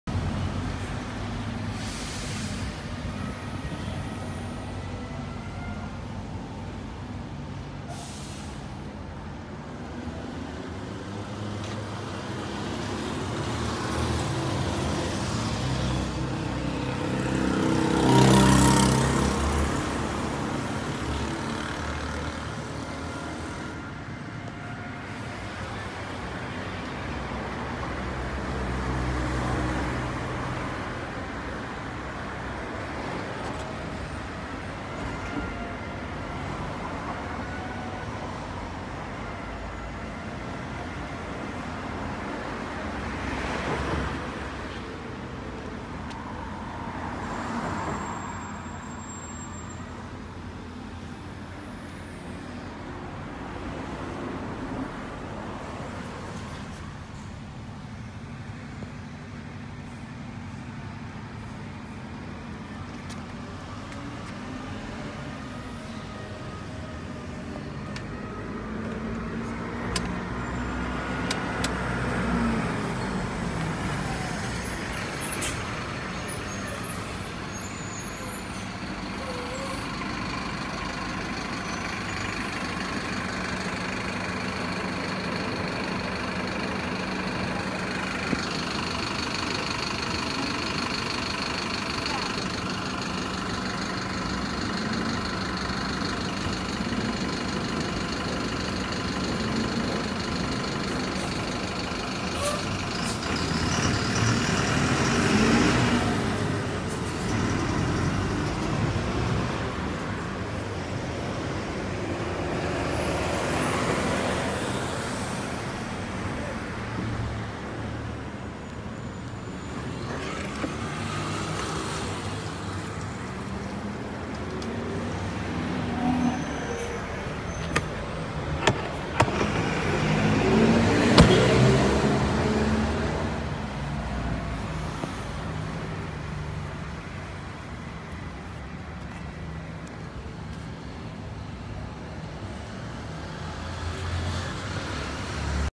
Kymco Downtown 300i ABS 2020
Kymco Downtown 300i ABS 2020 com APENAS 1.500km rodados, cinza, único dono com freio ABS, manual e chave reserva, moto impecável e sem detalhes!!!